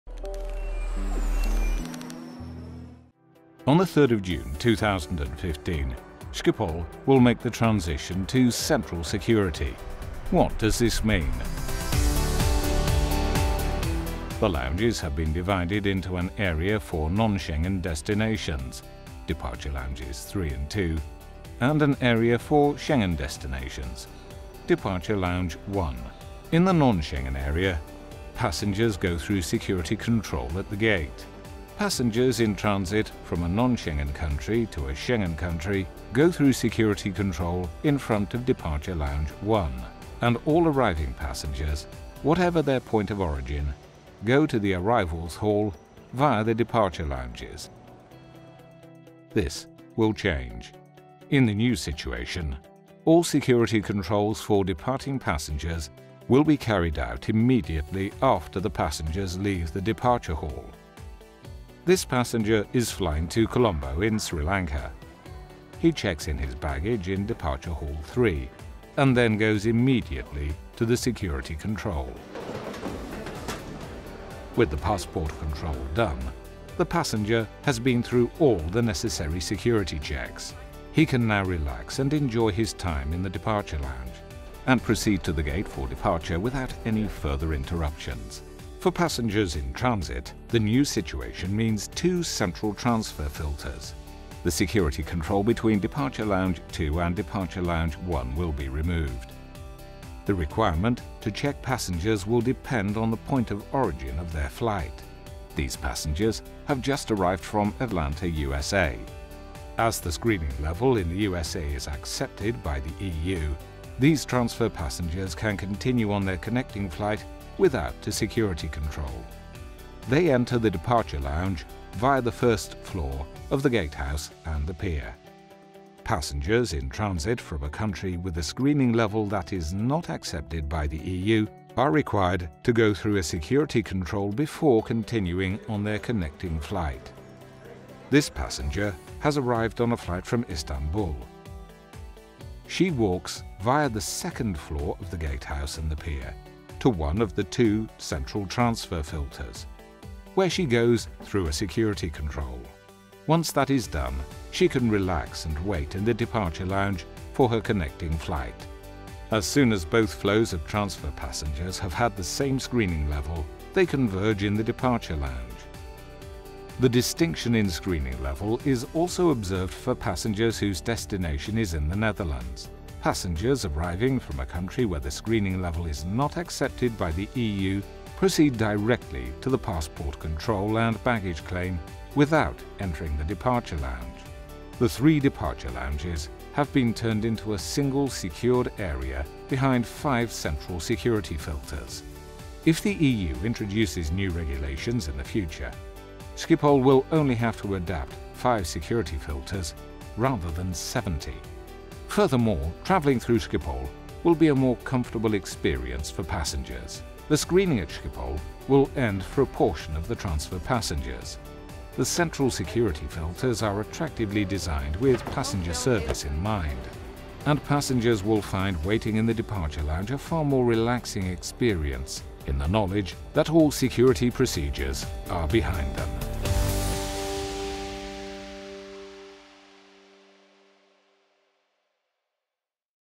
Central Security at Schiphol: how does it work?
Starting 3 June 2015, security checks for passengers and their hand baggage will no longer be carried out at the gates, but at central locations throughout the terminal. In Departure Halls 2 and 3, central security locations have been constructed for departing passengers. They are also referred to as 'security filters' and are located beyond the check-in desks. Security filters have also been built between the E and the F Piers and at the G and the H Piers for passengers who are transferring at Schiphol and are required to pass through security control again.